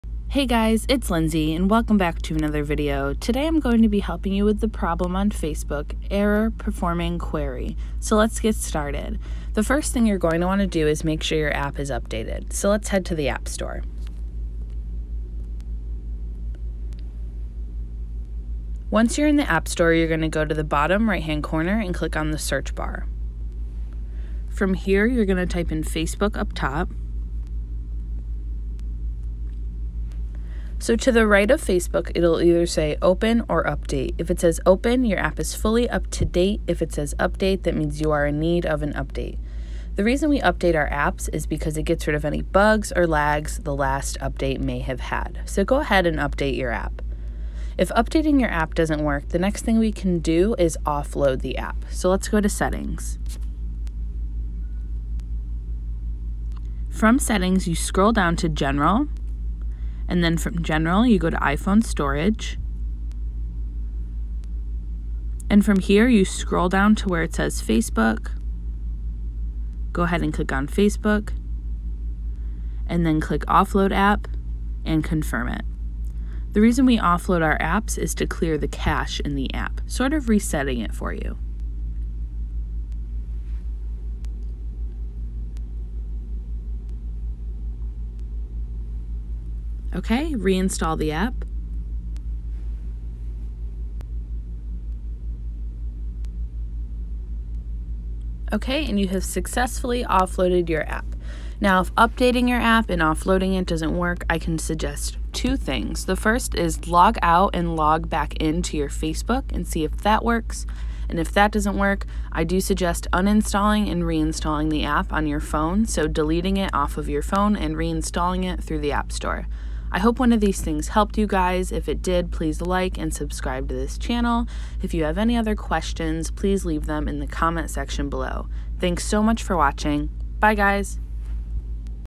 Facebook Error Performing Query (FIXed!)
Are you getting the Facebook message error performing query and are scratching your head to fix it? In this video, I walk you through several steps you can take to get Facebook working again. This is a common error people get with photos and events, basically, there is some issue connecting Facebook servers to the information you're looking for. Likely this is an issue with the Facebook servers or a bug in the app, so follow these steps to fix it. We've also seen this error associated with ads, workplace, and messenger. These fixes work on iPhone, Android, and PC.

I hope this video helped solve your problem. Do you still have a question about this app?